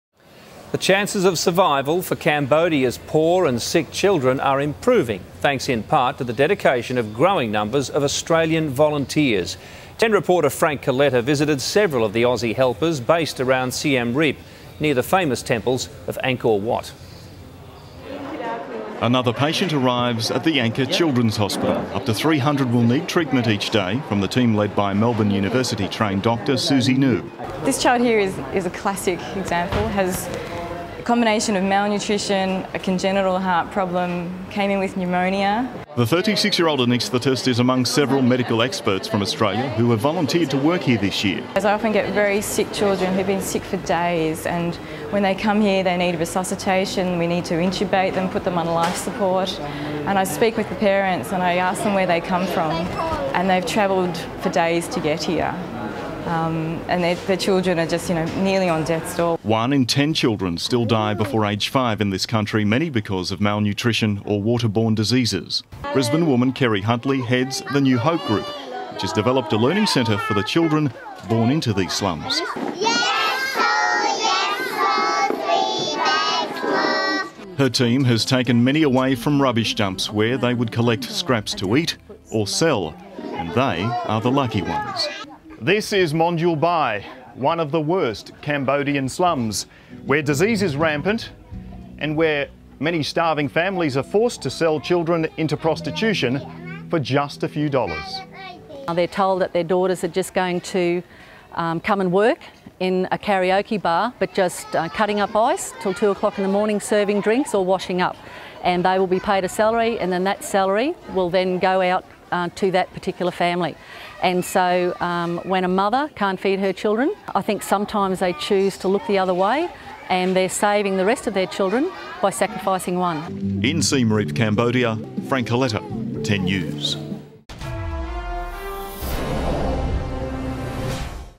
Healing Cambodia
The chances of survival for Cambodia's poor and sick children are improving, thanks in part to the dedication of growing numbers of Australian volunteers.